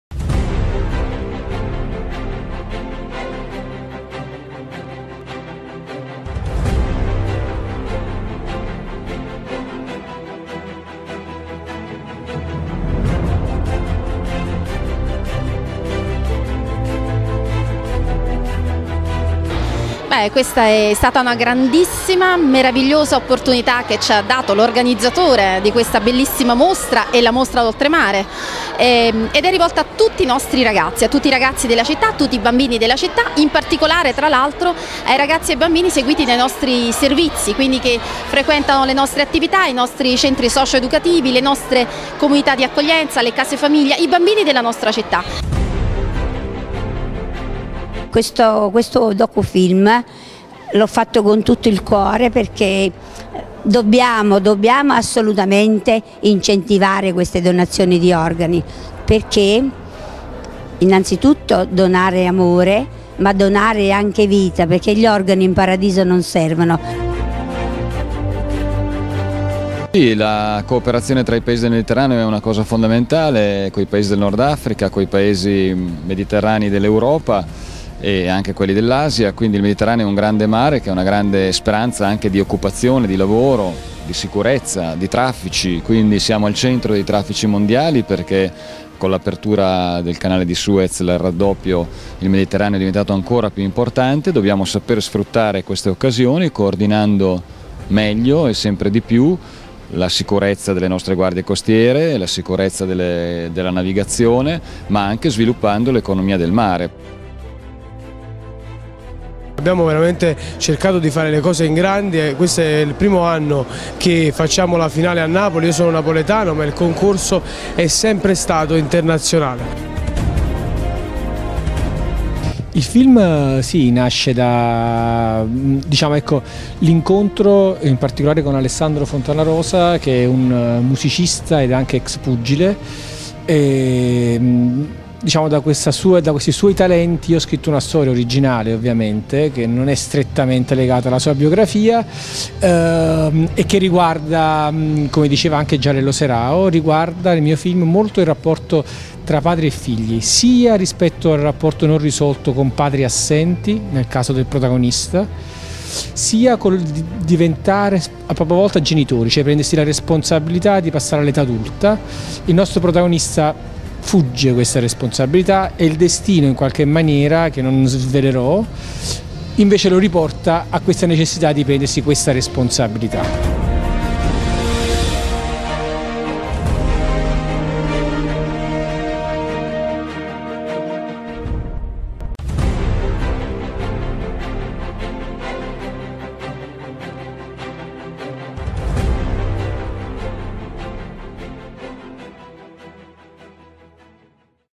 TG SINAPSI NEWS (5)
ALL’ INTERNO
- “Brick Live” a Napoli Mostra D'Oltremare

- DA MARIA A CHIARA UN DOCUFILM CHE TOCCA IL “CUORE”
di Antonella D'Agostino

- HFM  Stake Holder Conference. Napoli Castel dell’Ovo
ai Microfoni il Ministro Graziano Del Rio

- MISS EUROPA CONTINENTAL
Intervista al  patron di  Alberto CERQUA

- Presentazione del film CORE & SANG
di Lucio Fiorentino